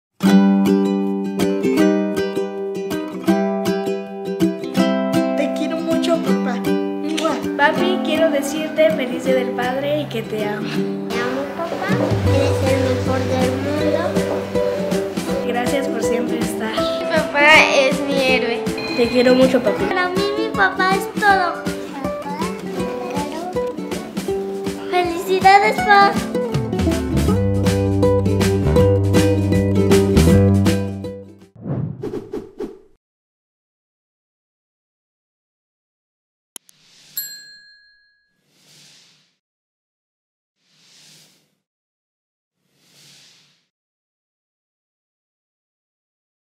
¡Feliz Día del Padre! 🎉
🎉 Este Día del Padre queremos celebrar a todos los papás con un mensaje muy especial. 👨‍👧‍👦

¡Hoy es un buen día para expresar todo tu cariño y gratitud! 💖 Comparte tu mensaje en los comentarios y hazle saber cuánto lo amas y aprecias.